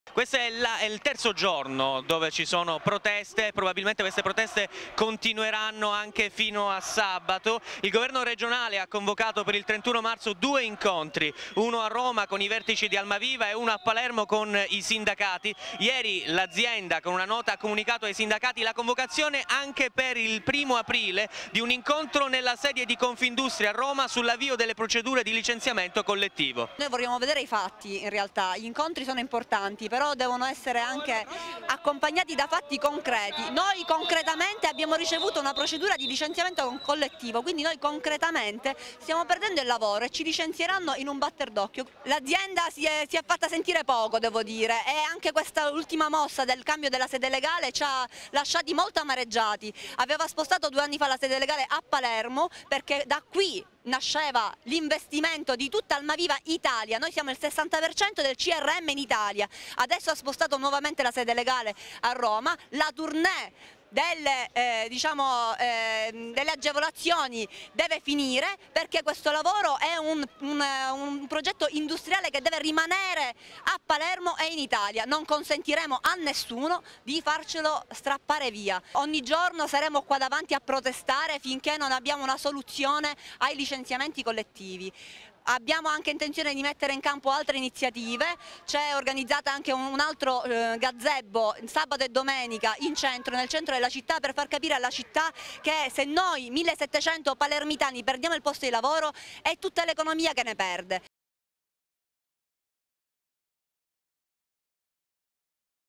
Almaviva, proteste e blocchi stradali in via Cordova [TgMed 23/03/2016]
Via Cordova bloccata dall'ennesimo presidio
dei lavoratori del call center Almaviva.
Continua la protesta contro i 1670
licenziamenti previsti a Palermo. Tra i lavoratori
non c'è molto ottimismo sugli incontri previsti
nelle prossime settimane.